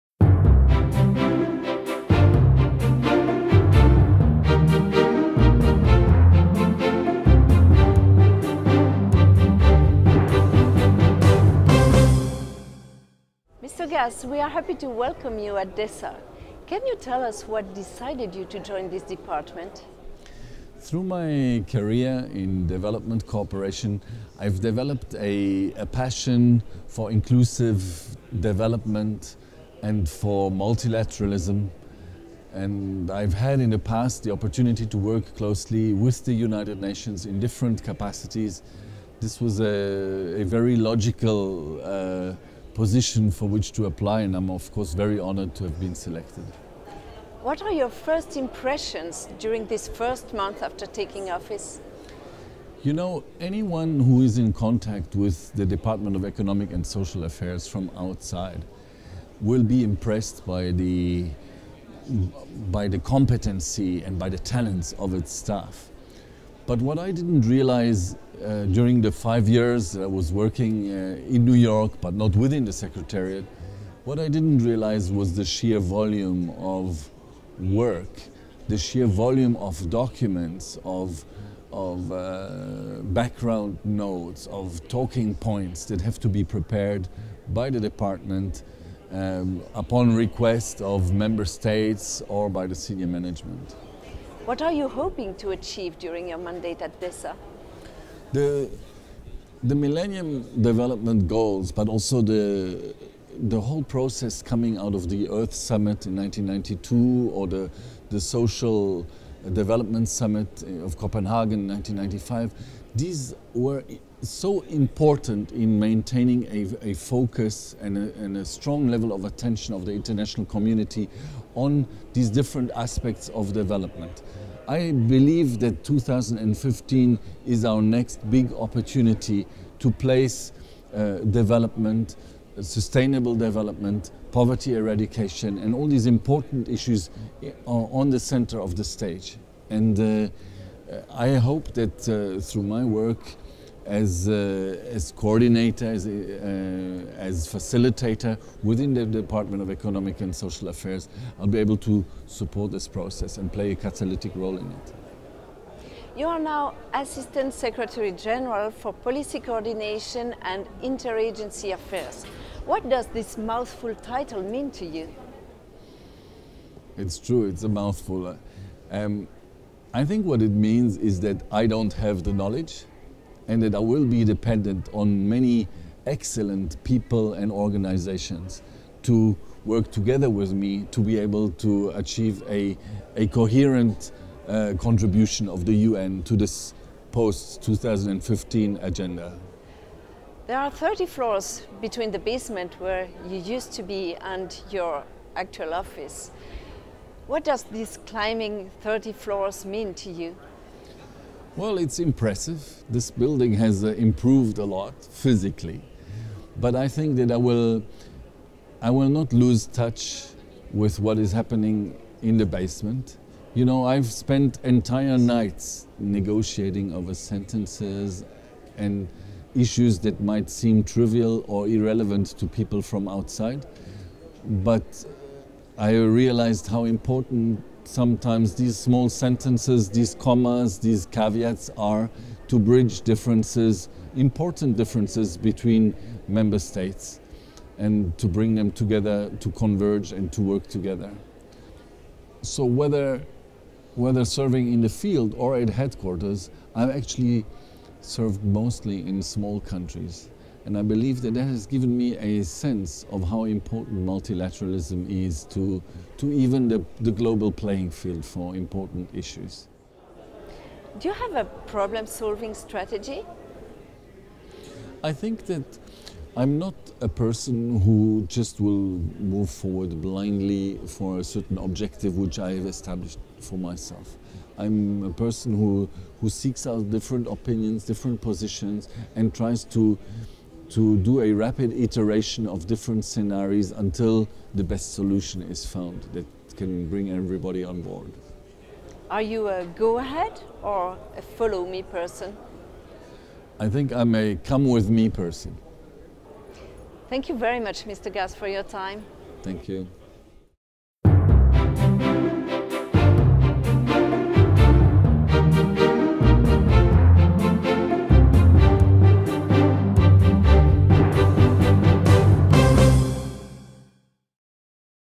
Interview with Thomas Gass, ASG of DESA - DESA News Oct 13
Thomas Gass, from Switzerland, was appointed by the Secretary-General Ban Ki-moon as Assistant Secretary-General for Policy Coordination and Inter-Agency Affairs in UN DESA. He took office on 3 September 2013. The new ASG describes himself as an unconditional supporter of inclusive development and multilateralism. In his video interview, he explains how his experience in development cooperation and as a Member State representative will be an added value to work on setting Sustainable Development Goals in the coming years.